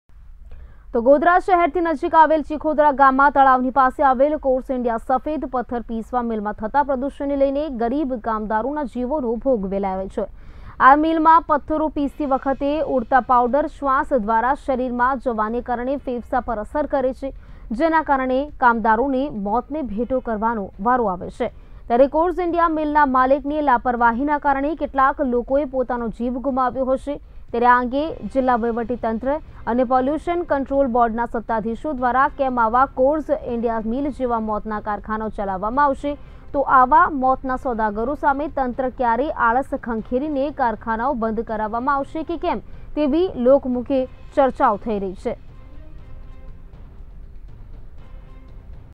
પંચમહાલ: મિલમાં થતા પ્રદુષણમાં લેવાઈ રહ્યો છે ગરીબ કામદારોના જીવોનો ભોગ, મિલના માલિકની લાપરવાહી
ચિખોદરા ગામના લોકોને જીવનું જોખમ
પથ્થર પિસવાનું કામ કરતા મજૂરોને જીવનું જોખમ
પ્રદુષણના કારણે ફેફ્સાંનું કેન્સર થવાનું જોખમ
કોર્સ ઇન્ડિયા સફેદ પથ્થર પીસવાની આવેલી છે મિલ
લાપરવાહીને કારણે અનેક મોત,તંત્ર ઘોર નિંદ્રામાં